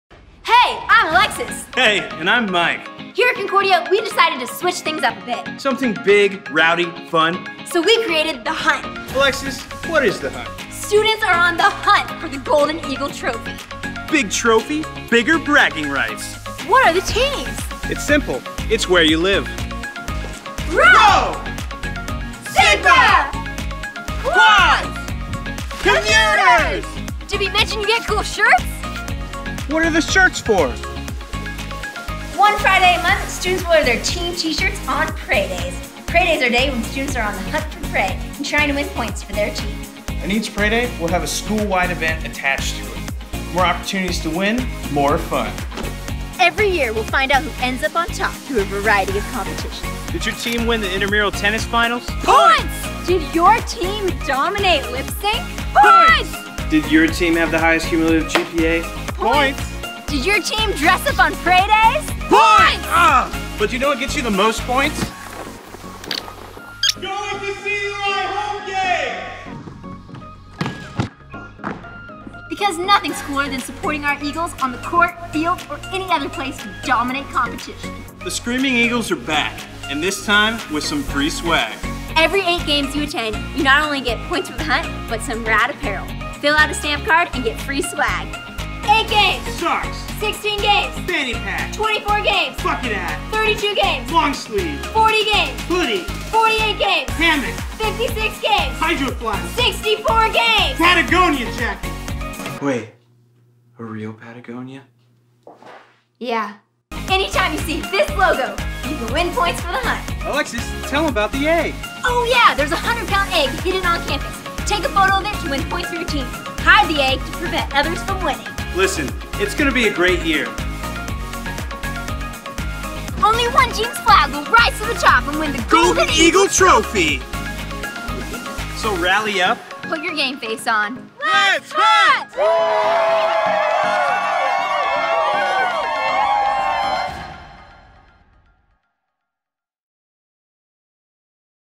What is The Hunt for the Golden Eagle?
Concordia University Irvine students are on The Hunt for the Golden Eagle! Students can participate in a series of spirit days, events, activities, intramurals, and compete in the Hunter Games at the end of the school year to win the ultimate prize of the Golden Eagle trophy and bragging rights for their community team.